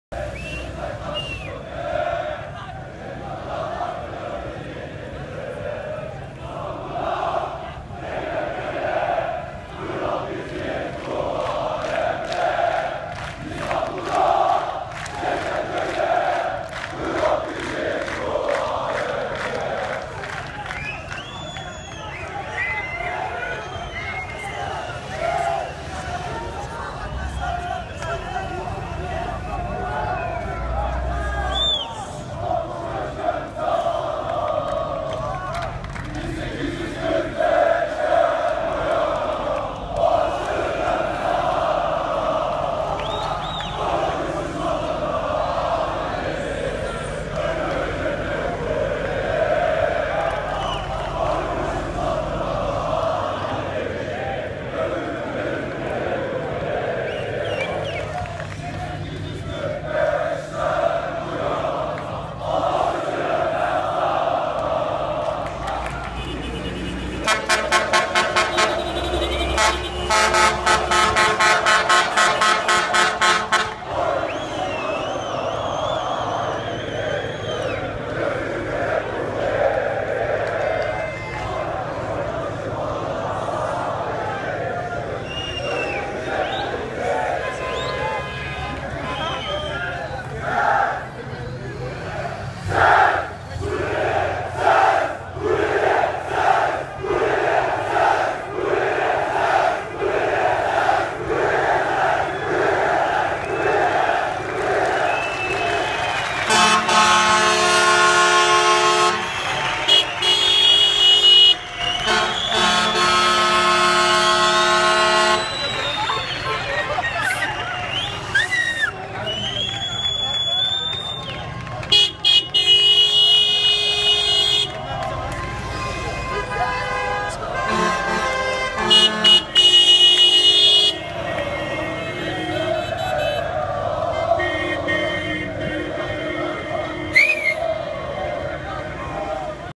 Kuleli Military School Students Comradery - Graduation 2014
An epic comradery among the Kuleli Military School students -- future Turkish soldiers... Freshmen and the senior students are signing together while senior soldiers are on a boat celebrating their graduation and the freshmen students are at the school building greeting and singing right back at their senior brothers. I was just passing in a car and made my friend stop the car to record this. This was amazing. So proud to be a Turk.